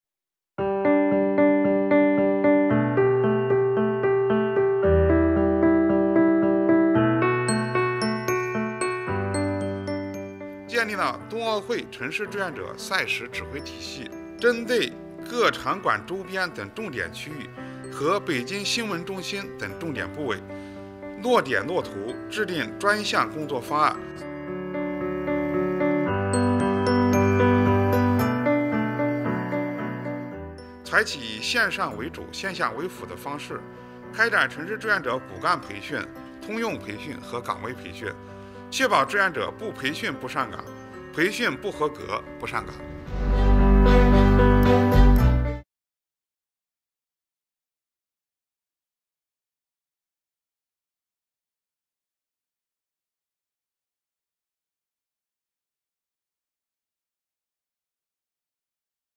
Beijing to Deploy 200,000 Volunteers to Serve Winter Olympics on Multifarious Duties
Beijing will deploy about 200,000 volunteers to serve the upcoming 2022 Winter Olympic and Paralympic Games in various aspects and venues on multifarious duties, said the volunteers' headquarters at a press briefing on Friday.